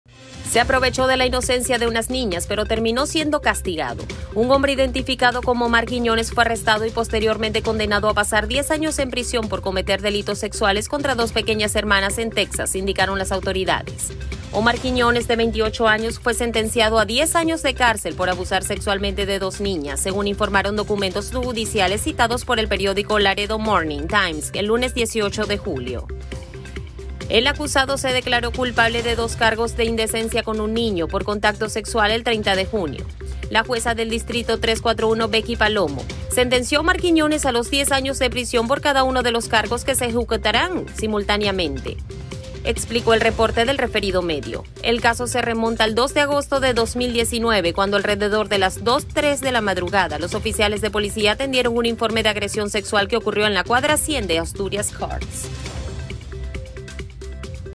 Hispano acusado por una agresión sexual contra dos hermanitas menores
Se aprovechó de la inocencia de unas niñas, pero terminó siendo ‘castigado’. Un hombre identificado como Omar Quiñones fue arrestado y posteriormente condenado a pasar 10 años en prisión por cometer delitos sexuales contra dos pequeñas hermanas en Texas, indicaron las autoridades.

Omar Quiñones, de 28 años, fue sentenciado a 10 años de cárcel por abusar sexualmente de dos niñas, según informaron documentos judiciales citados por el periódico Laredo Morning times el lunes 18 de julio. El acusado se declaró culpable de dos cargos de indecencia con un niño por contacto sexual el 30 de junio.